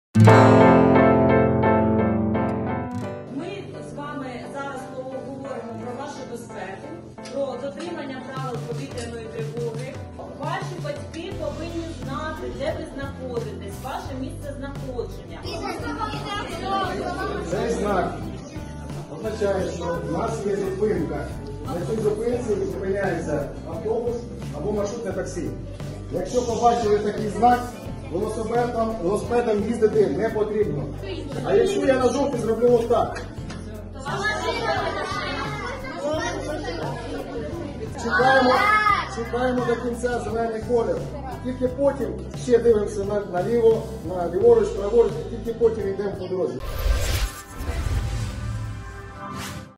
Мобільний клас безпеки продовжує свою подорож
Мобільний клас безпеки продовжує свою подорож: криворізькі поліцейські навчали дітей правилам безпечної поведінки

Перевагою цього класу є мобільність, яка дозволяє швидко змінювати локації, аби познайомити більшість школярів з правилами безпеки.

1 грудня ювенальні поліцейські та дільничні, спільно з рятувальниками завітали до учнів Глеюватської громади Криворізького району, що обслуговується відділенням поліції №3 Криворізького РУП.

За допомогою мобільного класу безпеки, в якому розміщені різноманітні макети небезпечних предметів та наочні матеріали, правоохоронці познайомили дітей з мінною, пожежною та побутовою безпекою.

Крім того, школярам нагадали правила дорожнього руху, ознайомили з дорожніми знаками та пояснили чому так важливо мати світловідбиваючі елементи на одязі у темну пору доби.

Також поліцейські розповіли алгоритм дій під час повітряної тривоги.

Насамкінець зустрічі малеча мала змогу відчути себе справжніми поліцейськими та рятувальниками, примірявши формений одяг.

Відділ комунікації поліції Дніпропетровської області
за матеріалами Криворізького районного управління поліції